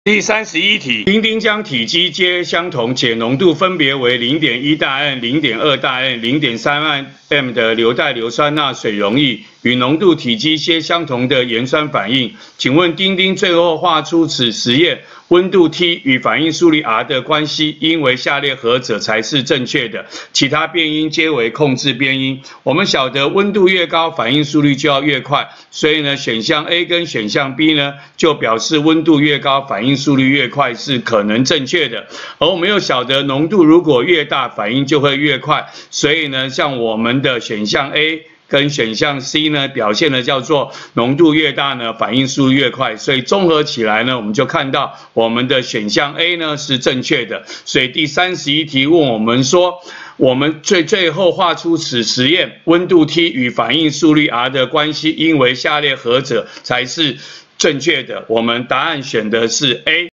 107學年下8年級理化二段第31題
不同濃度硫代硫酸鈉水溶液與濃度、體積皆相同的鹽酸反應，請問實驗溫度(T)與反應速率(R)的關係圖為何。